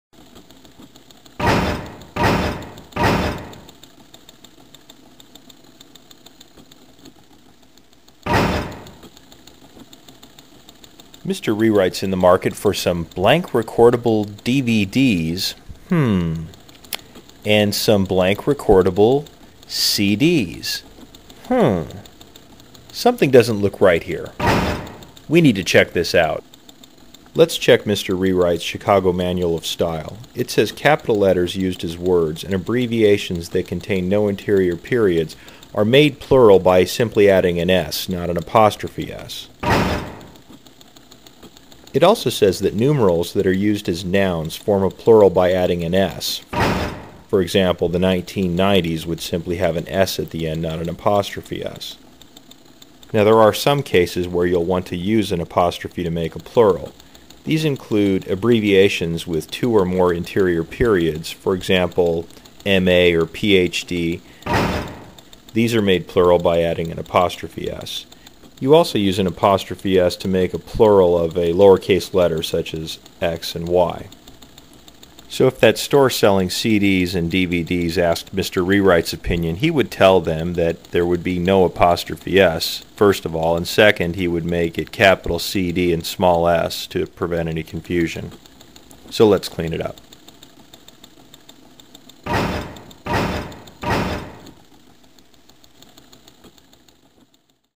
Mr. Rewrite: DVD's or DVDs?
Hijinks ensue when Mr. Rewrite spots a store selling DVD's and CD's.